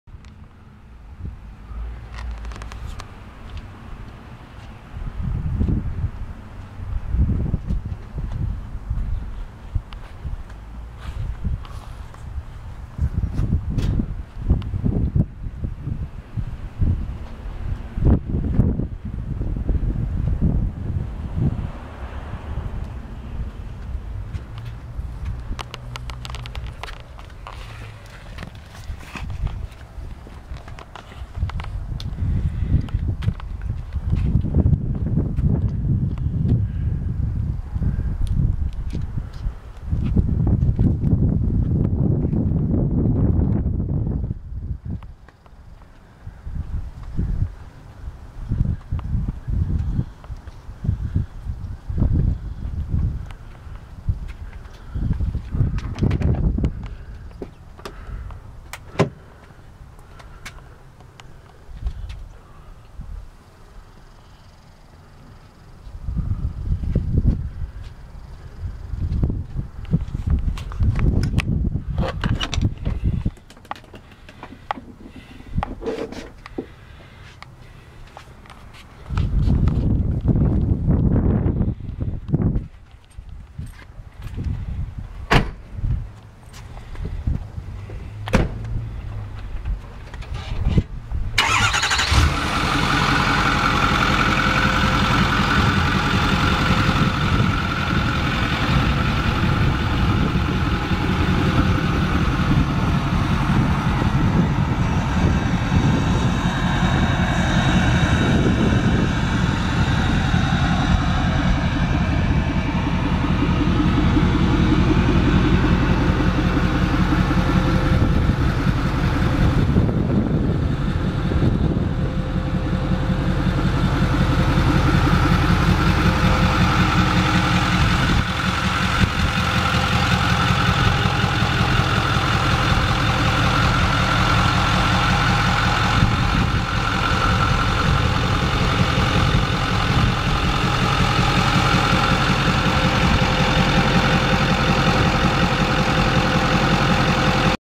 2008 FORD F-350 DUALLY SOLD SOLD SOLD @ ERICS MUSCLE CARS
H&S mini race tuner extra 350 hp est. 650total & about 1000lbs torque
18" KMC Rockstar XD wheels
Satellite radio sirius xm
Ads 91GAL. fuel tank and toolbox
Flo pro 5 inch dp back DPF & CAT delete with a 6" tip.
Full Egr del.
Stainless steel Intake elbow by flopro
S&B stage 2 cold air intake
All chrome plastics around rear lights mirrors handles etc by car ID
All lights by recon.
1 brake over rear window
2 tailgate light
3 amber running board lights
Still have all oem products except for wheels.